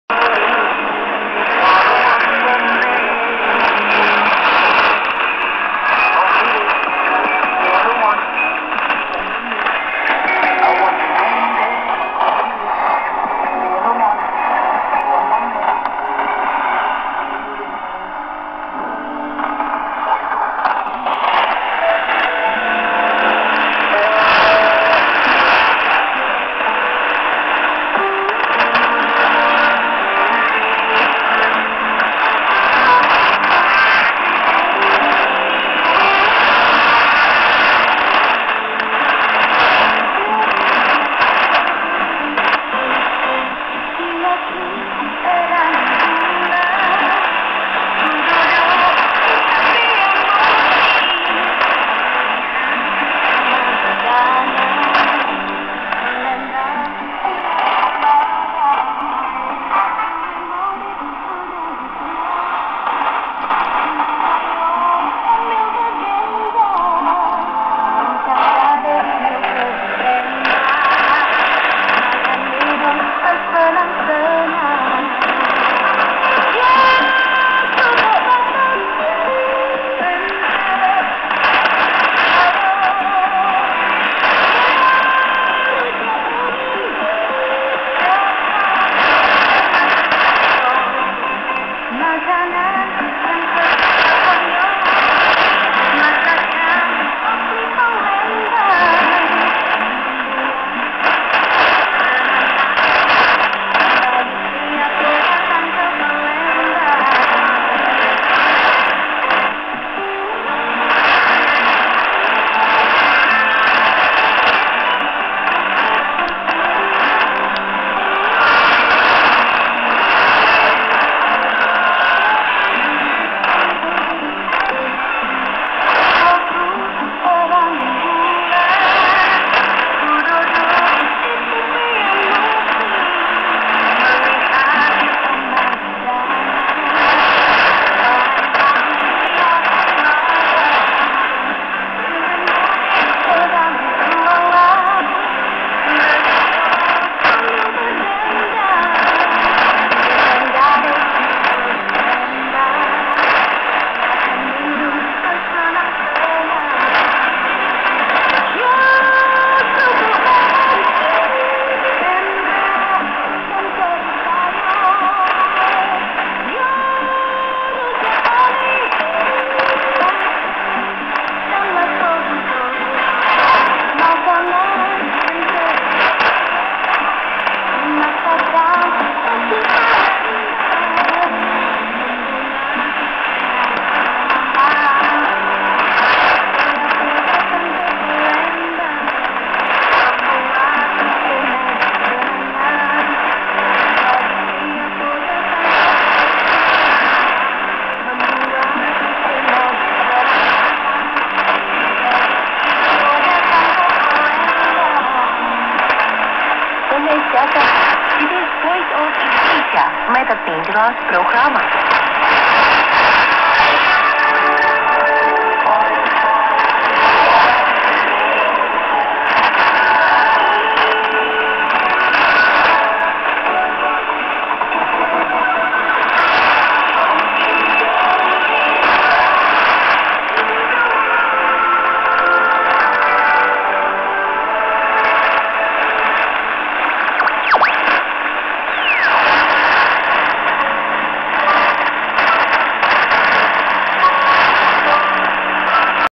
RRI WS (Voice of Indonesia) 3325 kHz
This is VOI's 10 kW transmitter 3:25 from Palangka Raya, Indonesia with English program. Received at 2:00 during G1 geomagnetic storm on May 26th 2021 in Northwestern Siberia of Russia.